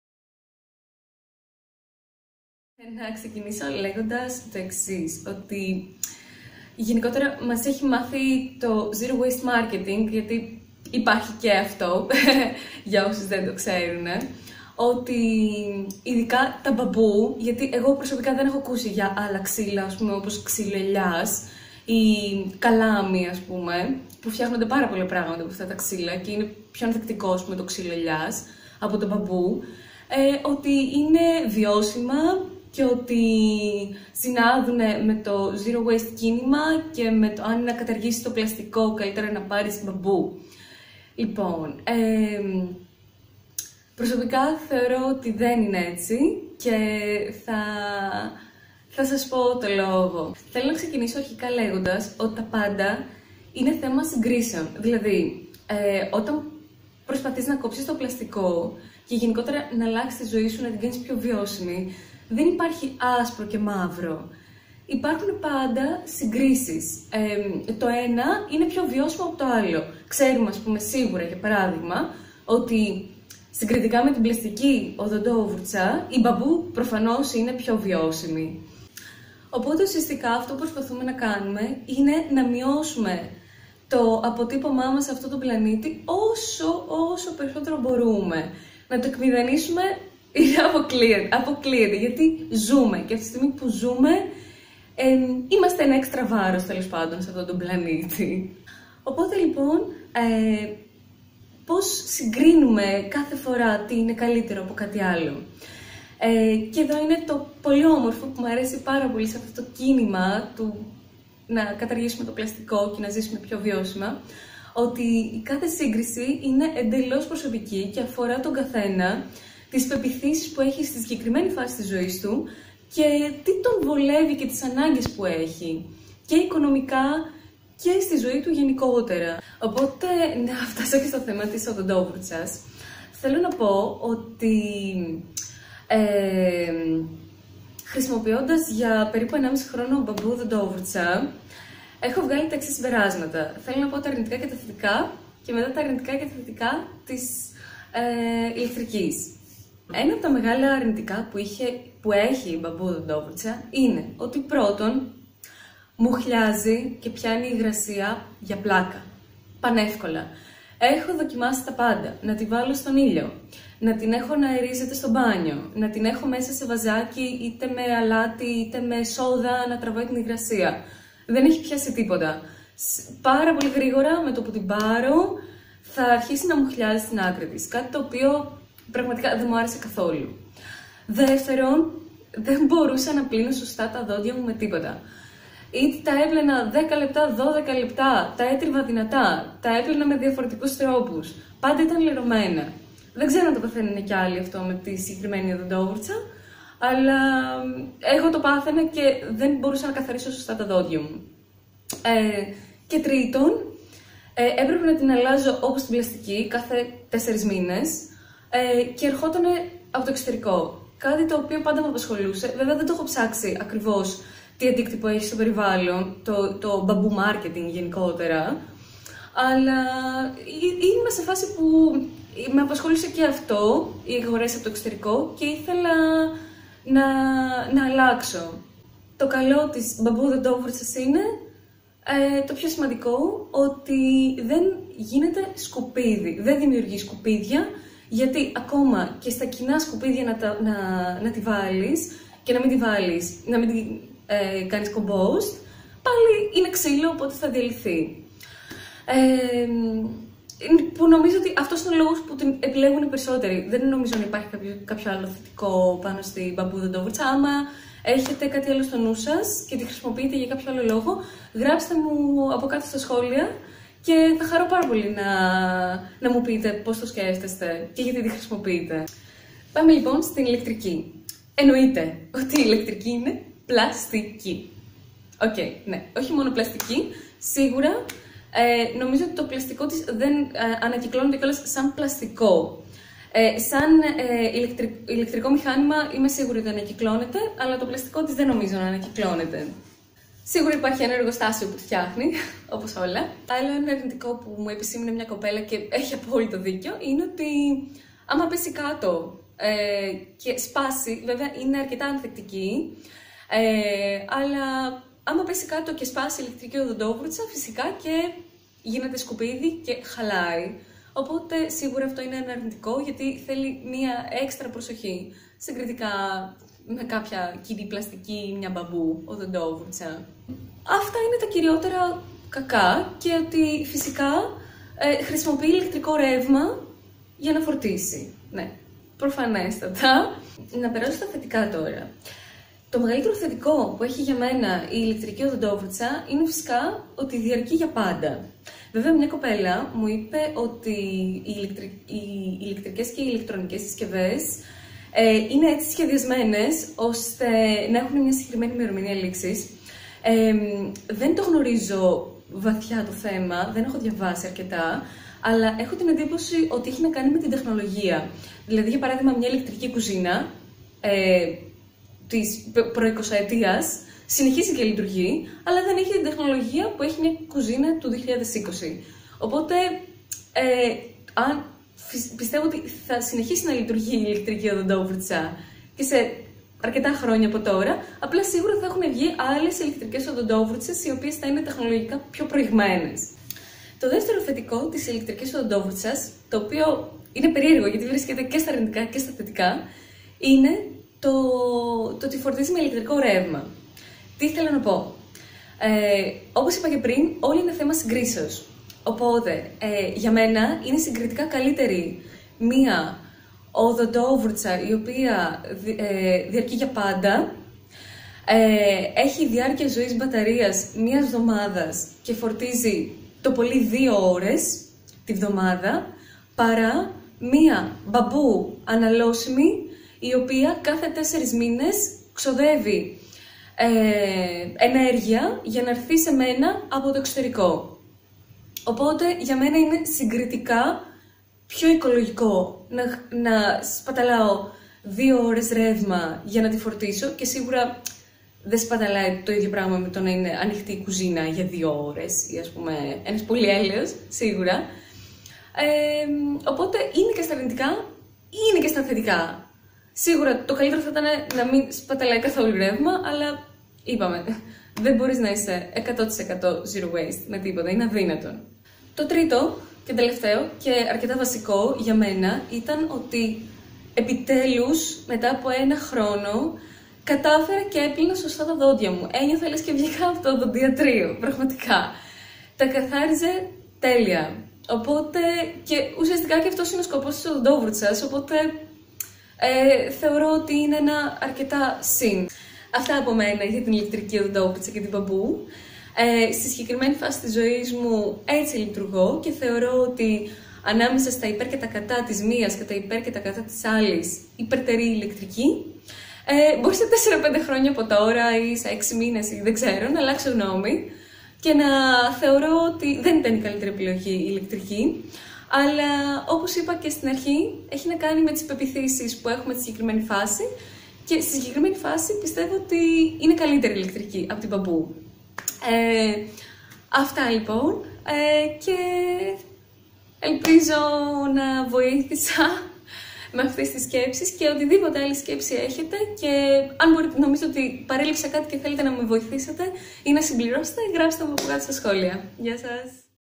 BAMBOO vs. ELECTRIC TOOTHBRUSH:Γιατί ΔΕ χρησιμοποιώ μπαμπού οδοντόβουρτσα;/ZERO WASTE SWAPS
*Scroll down fro English*

Σε αυτό το βίντεο συγκρίνω τη μπαμπού οδοντόβουρτσα με την ηλεκτρική!Ποια είναι πιο βιώσιμη;Πώς επιλέγω κάθε φορά τι είναι πιο βιώσιμο απο κάτι άλλο;
Aκολουθήστε με στο Instagram όπου δίνω καθημερινά πολλές συμβουλές πάνω στο μινιμαλισμό,στο zero waste και στο βιώσιμο τρόπο ζωής και μοιράζομαι μαζί σας στιγμιότυπα από την καθημερινότητα μου!
In this video,i compare bamboo and electric toothbrush!Which one is more sustainable?How do i choose everytime what is more sustainable?